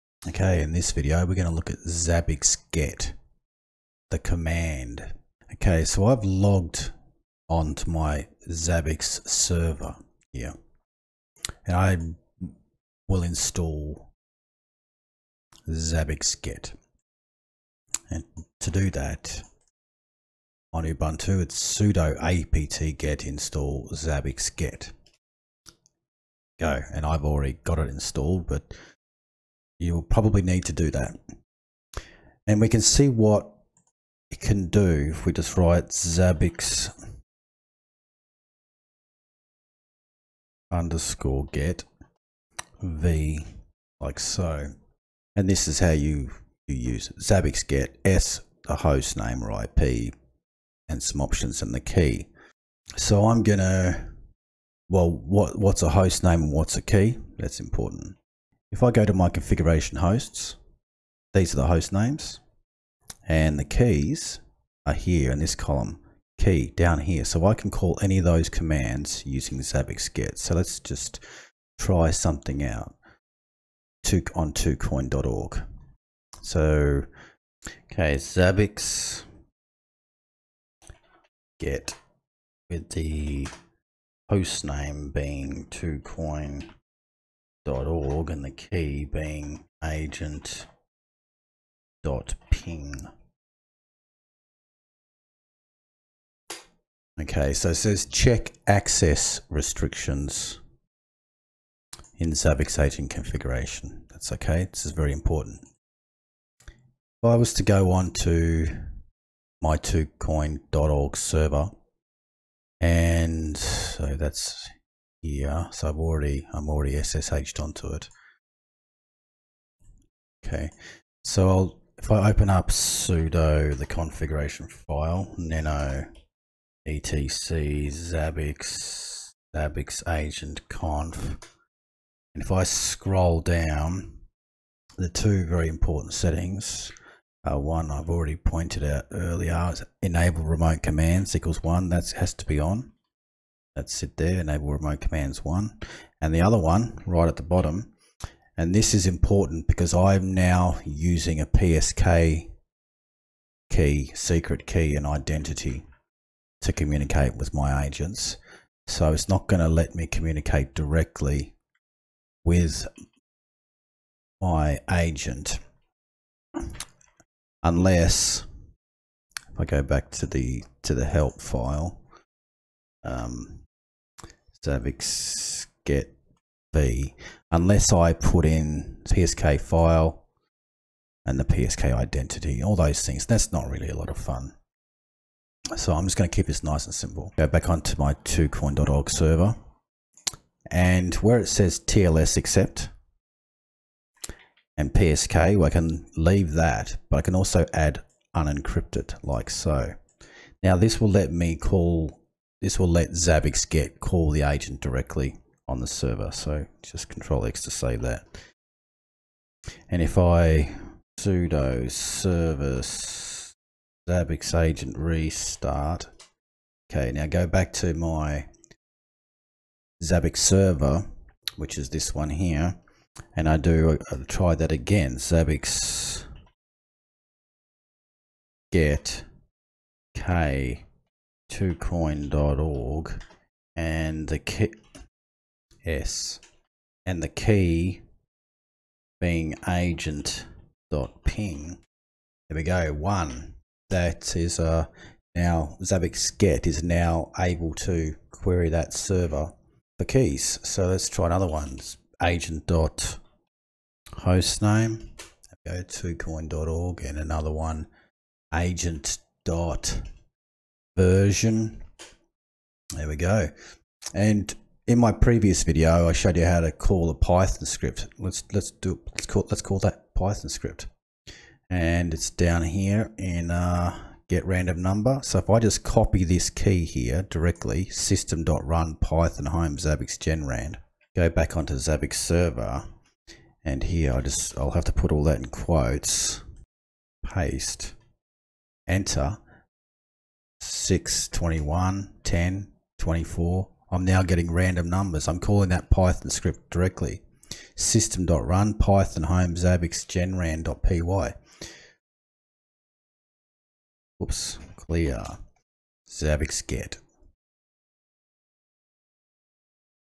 Using the 'Zabbix Get' 4.2 Command
Zabbix-get is a command line tool that allows you to manually query keys on the remote hosts.

To install on ubuntu,

$ sudo apt-get install zabbix-get

For help use

$ zabbix_get -h

Ensure that EnableRemoteCommands=1 in zabbix-agentd.conf and if you are using psk encryption you will need to also enter the psk information, or add 'unencrypted' to the options for TLSAccept in the agents configuration file.